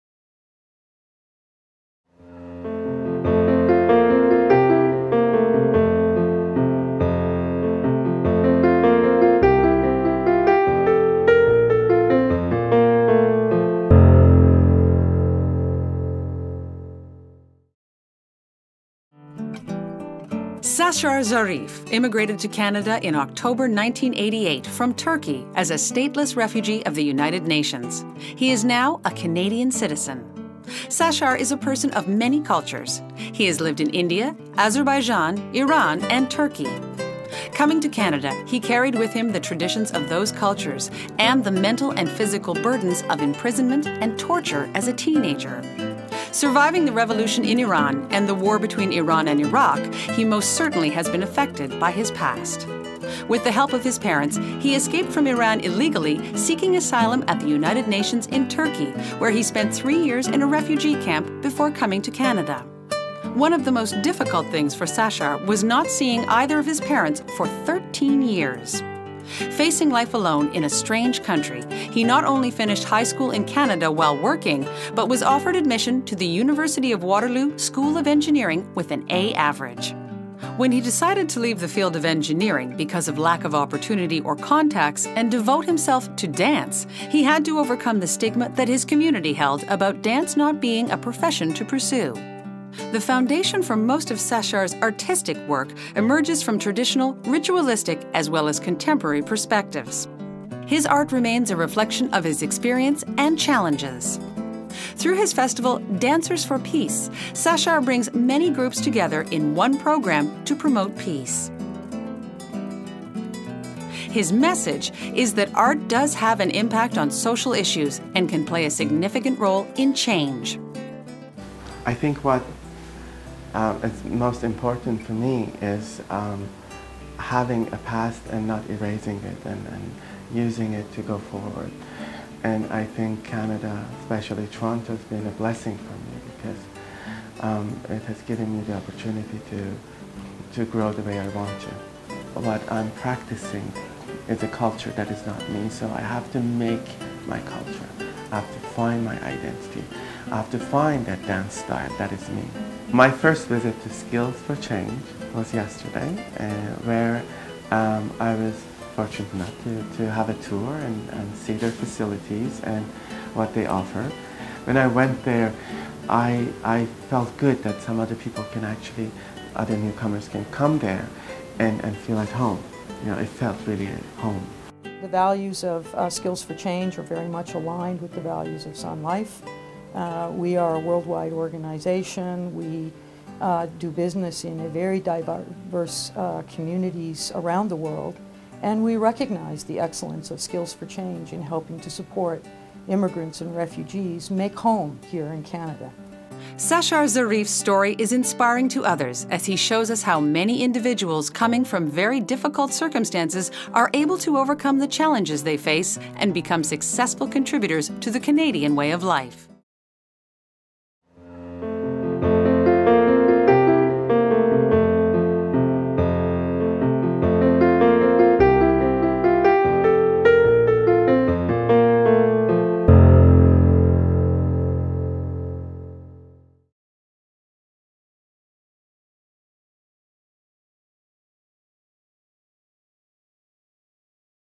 2008 New Pioneers Awards SfCarchive
In 1988, Sashar Zarif immigrated to Canada from Turkey as a Stateless Refugee of the United Nations. With the assistance of his parents he had escaped from Iran through the mountains and sought asylum in Turkey. After three years in a refugee camp in Turkey, he arrived in Canada alone. One of the most difficult things about immigrating to Canada was not seeing either of his parents for 13 years.

Sashar has lived in many different countries, including India, Azerbaijan, Iran and Turkey. He arrived in Canada carrying the traditions of those cultures and bearing the mental and physical burdens of imprisonment and torture he had witnessed and suffered as a teenager.

In Canada, Sashar faced financial hardship and isolation. Despite being alone in this country, he managed not only to make up - while working - the three years of schooling he had missed, but finished high school with an A average. He was then offered admission to a very prestigious engineering school - the University of Waterloo.

But in the end, engineering lost out to dance. For as long as he can remember, Sashar has been a dancer -- and an activist. As an artist, he has a commitment to "contribute to social and individual expression concerning pressing cultural issues: issues that impact the health of our society, our environment and our individuality -- including the well-being of our souls." His belief that the artist can create works that "express and promote the human urge towards living in harmony" has led him to extensive research and field work in dance and music ethnology throughout Central Asia, Eurasia and the Near East. Through teaching and performing - from Mongolia in the east to Europe and across South and North America - he has widely reflected and represented Canadian diversity.

Eighteen months after arriving in Canada, Sashar received an award from the East York Board of Education for his contribution to multiculturalism and diversity at Eastern High School of Commerce, where he was finishing his schooling. In 1993, he founded and directed the Sashar Zarif Dance Theatre and the Canadian Academy of Azerbaijani Dance. In 2001, Sashar founded, and continues to direct, Dancers for Peace, an international festival reflecting a vision of hope and peace that transcends barriers between nations, races and religions. In 2003, he was awarded the Chalmers Professional Development Grant and in 2004, he received his MA from York University, Faculty of Fine Arts, in Dance and Dance Ethnology. That same year, he began teaching at York University with the Dance Department, Faculty of Fine Arts, where he started a successful credit course called "Introduction to World Dance Practices: Dances of North Africa, the Middle East and Central Asia".

Sashar's message - that art can have an impact on social issues and play a significant role in change - resonates with the community at large. It is vital that newcomers insert themselves into their new culture in a positive and affirming way. Sashar leads by example; as an artistic ambassador for multiculturalism and diversity, he is a valued role model for new Canadians who have been through horrific experiences.